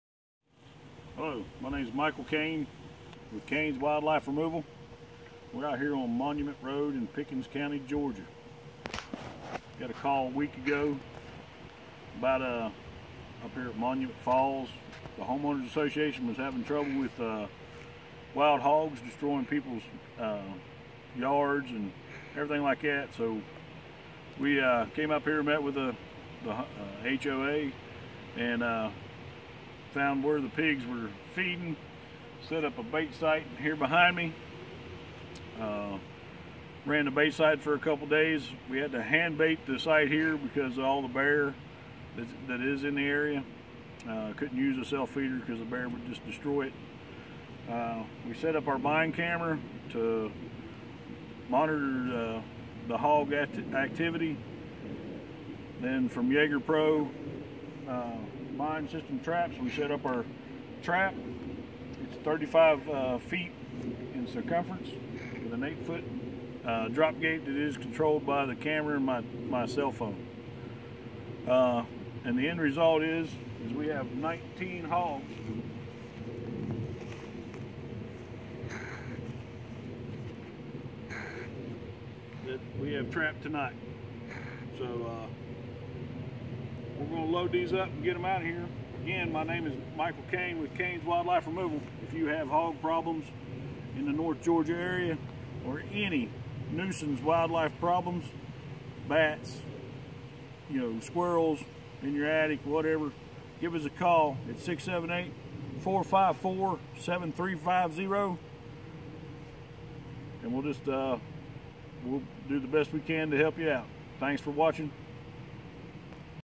Hog Trapping
Trapping 19 hogs in North Georgia